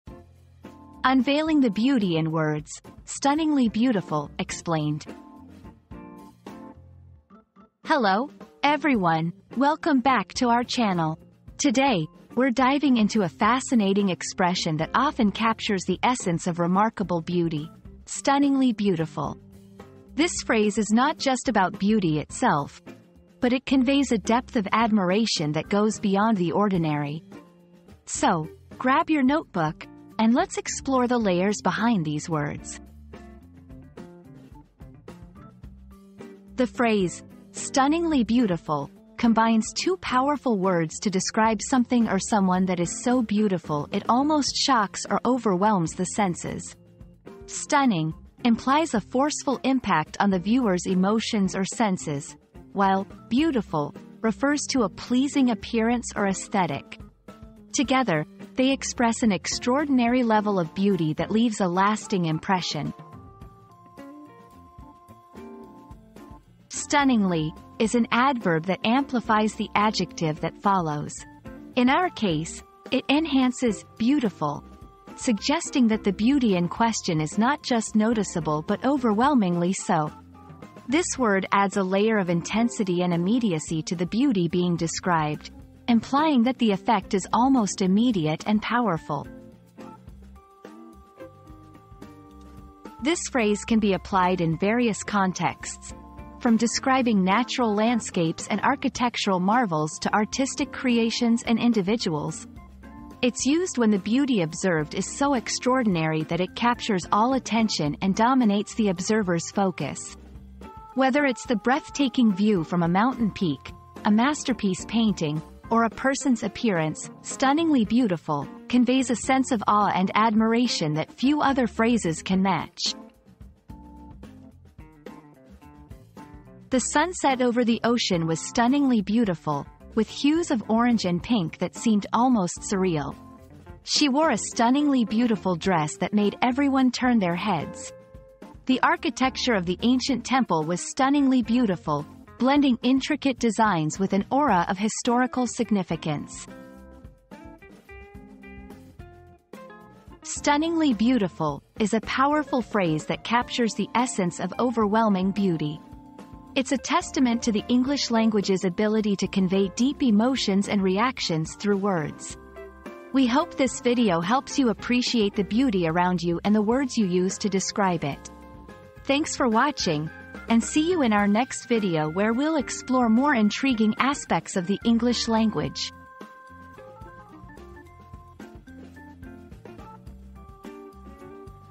Unveiling the Beauty in Words: "Stunningly Beautiful" Explained
Unveiling the Beauty in Words: Exploring 'Stunningly Beautiful' • Join us as we delve into the meaning and impact of the phrase 'Stunningly Beautiful' in this enlightening video. Discover the power and beauty of language in expressing emotions and experiences.

00:00 • Introduction - Unveiling the Beauty in Words: "Stunningly Beautiful" Explained
00:36 • Understanding "Stunningly Beautiful"
01:06 • The Impact of "Stunningly"
01:32 • Contexts Where "Stunningly Beautiful" Shines
02:06 • Examples in Sentences